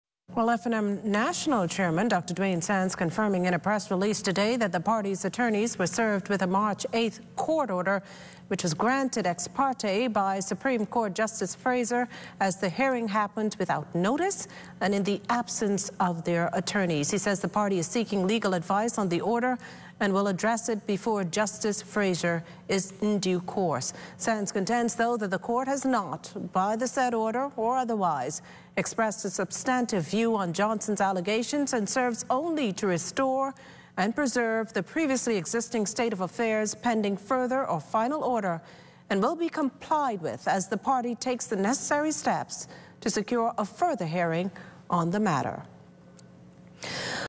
FNM Chairman Responds To Court Order
National Chairman of the Free National Movement confirms receipt of court order.